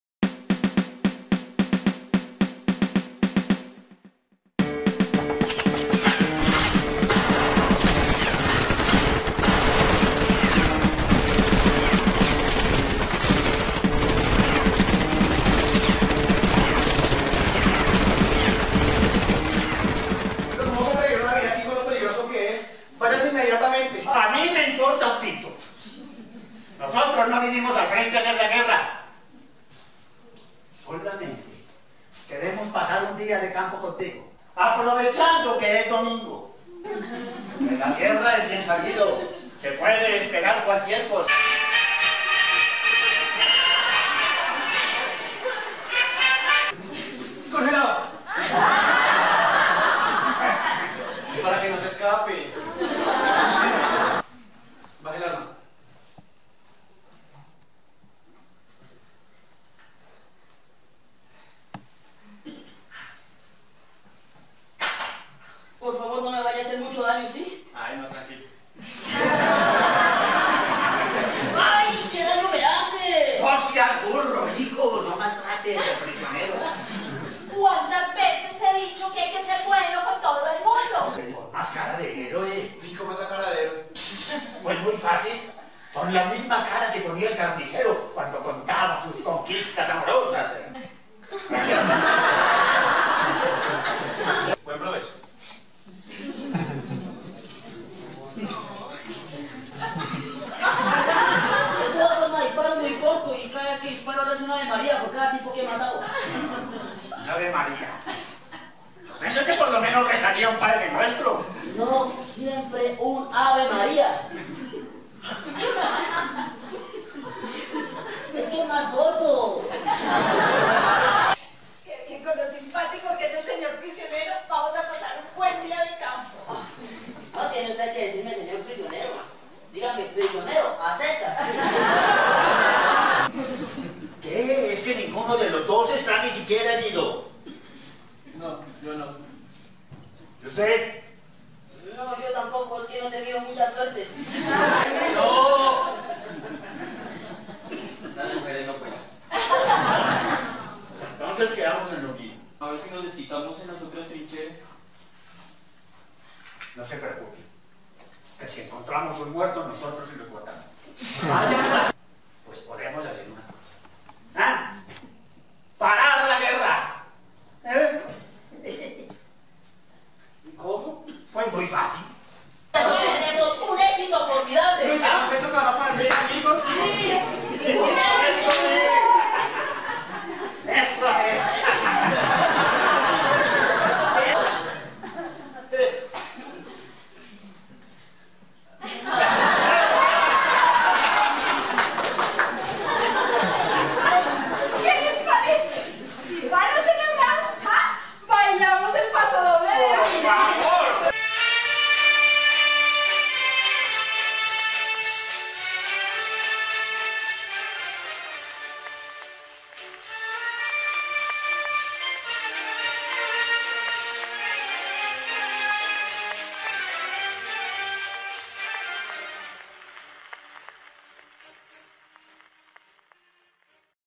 PIC-NIC de Fernando Arrabal
LA OBRA:
Es una divertida farsa llena de situaciones absurdas, que muestra el sin sentido de la guerra y como el poder colectivo oprime al individuo, impidiéndole su autonomía y la posibilidad de vivir.

SINOPSIS:
A los padres de un soldado que está en la guerra, se les ocurre ir un domingo hacer un picnic con su hijo en medio del campo de batalla. La llegada de un soldado enemigo, que termina siendo invitado a comer, las coincidencias entre los dos soldados, que definitivamente no quieren estar allí, los llevará a decidir no seguir el juego de la guerra, lo cual tendrá una inesperada consecuencia.

Bogotá - Colmbia